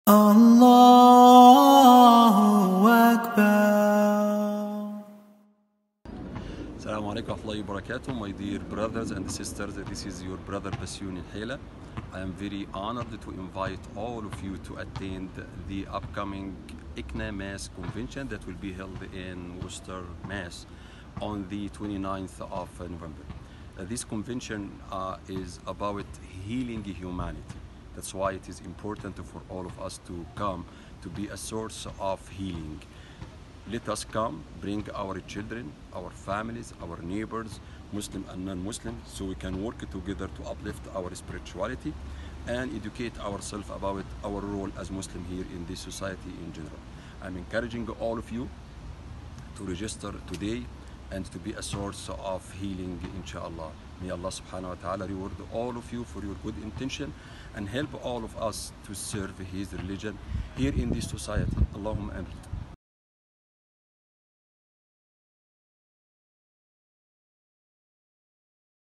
Dr. Basyouny Nehela invites YOU to 2019 NorthEast Regional Conv, Nov. 30-Dec.1
The ICNA-MAS NorthEast regional convention is being held on Nov. 30 - Dec. 1, 2019, in DCU Convention Center Worcester, MA.